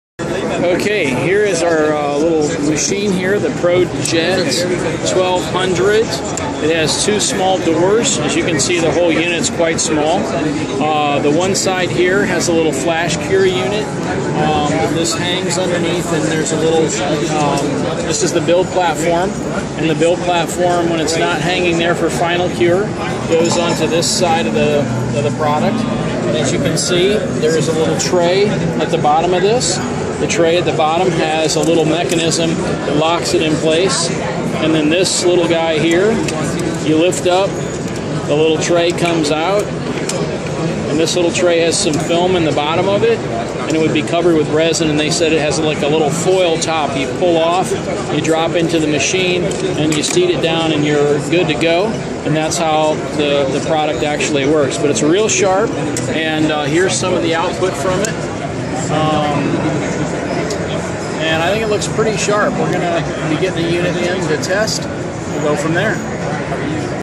ProJet 1200
Компактный 3D принтер, работающий по технологии SLA (стереолитография) для стоматологических лабораторий и ювелиров;
Готовые к использованию небольшие детали и шаблоны для литья по цене менее 1$;
Размером меньше, чем большинство кофемашин, изготовление детали занимает меньше времени, чем выпечка пирога;
Встроенный картридж с материалом VisiJet®FTX, подходящий для печати прототипов и изготовления шаблонов для литья.